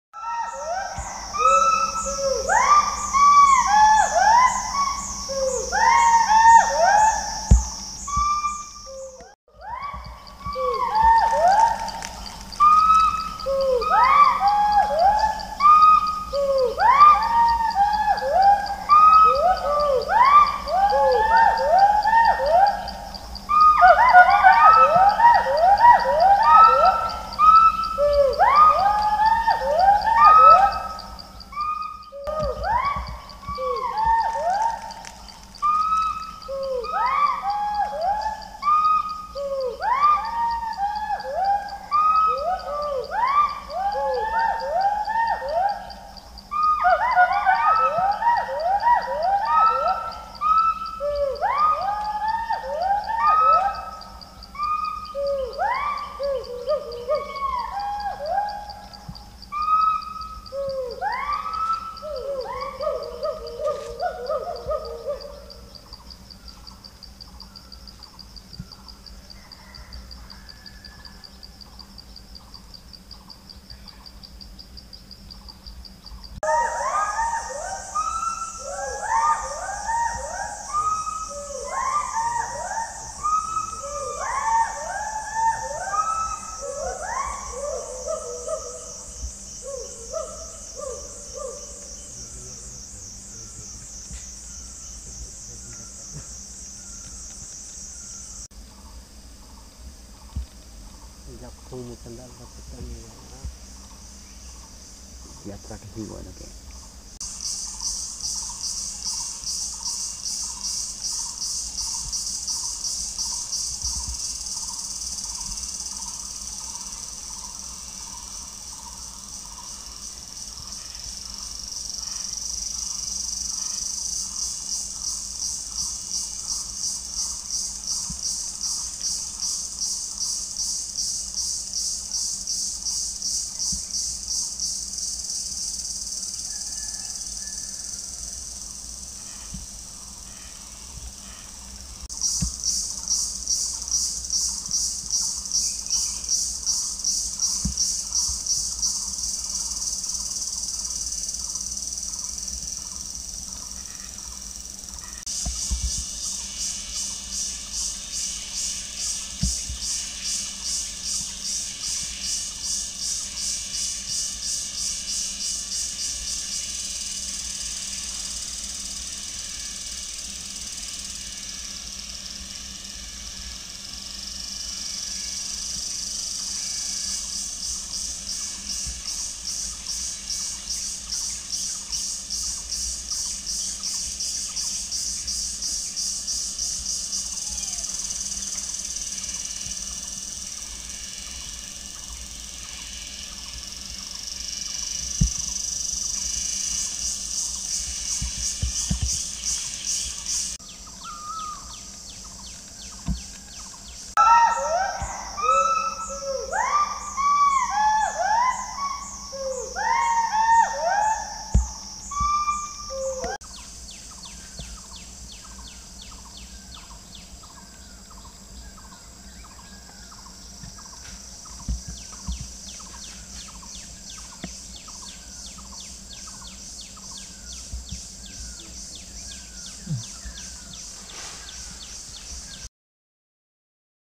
A'chik A'songni Huro (Hoolock Gibbons)
Ian South Garo Hills-ni song damsa Dabalgre songni Huro (Hoolock Gibbons) ong'a.